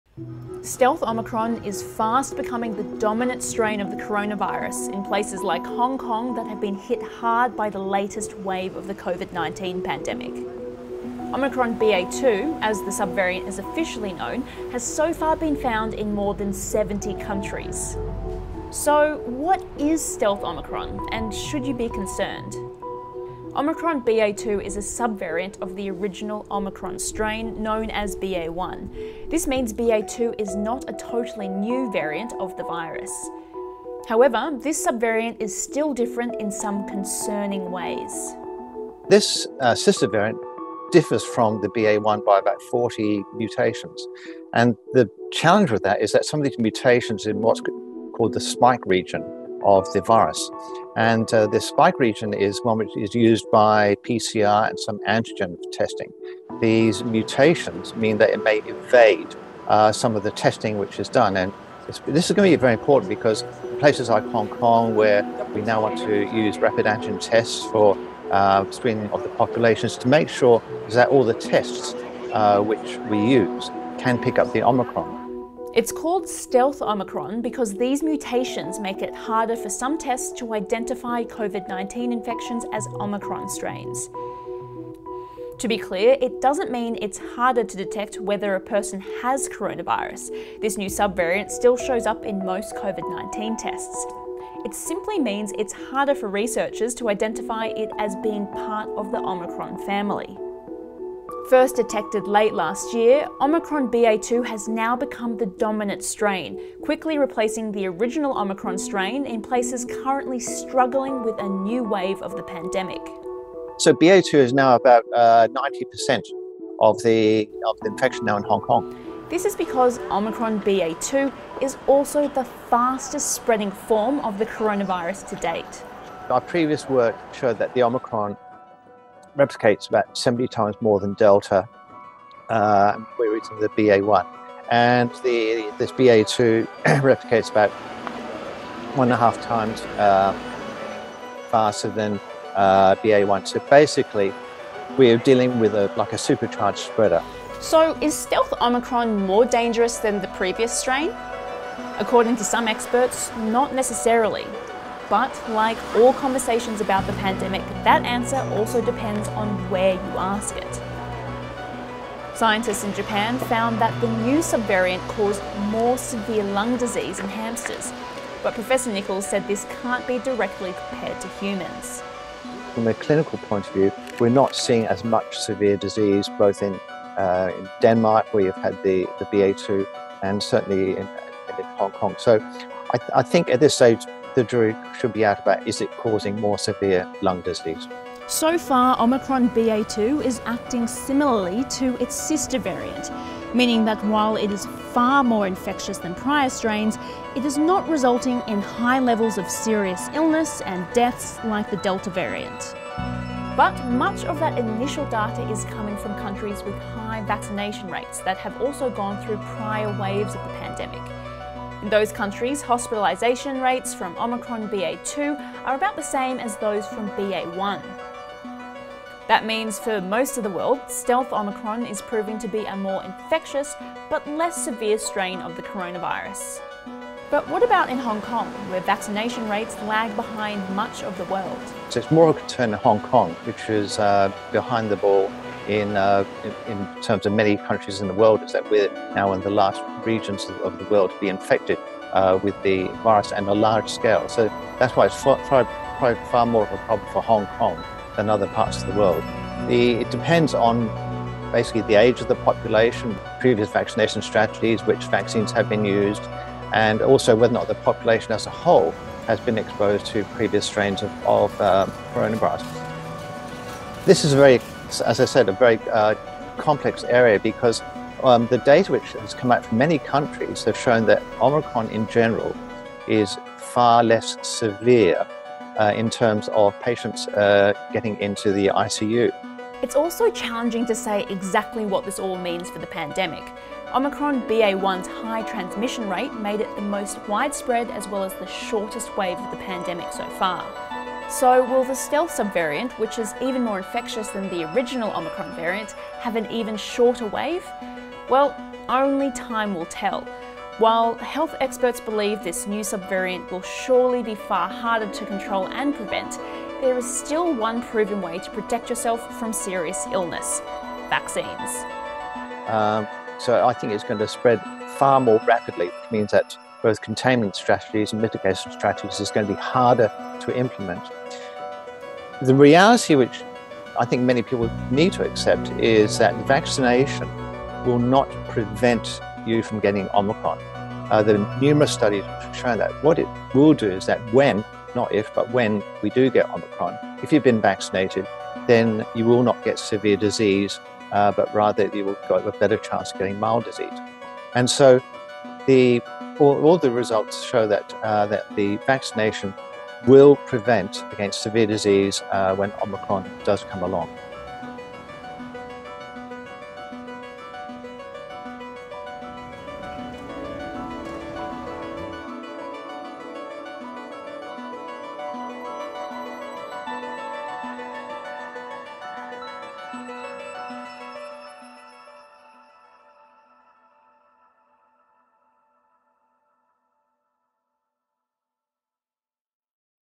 ‘Stealth Omicron’: How dangerous is the pandemic’s new highly infectious strain of Covid-19?
Omicron BA.2, a sister variant of the fast-spreading Omicron BA.1 variant, is proving to be an even more infectious form of the new strain of the Covid-19 virus. In Hong Kong, Omicron BA.2 quickly overtook BA.1 to become the dominant strain, fuelling the largest wave of cases seen in the city so far during the pandemic. But is Omicron BA.2 more dangerous than previous strains of the coronavirus? And why is it called “stealth Omicron”? The SCMP’s Alkira Reinfrank explains.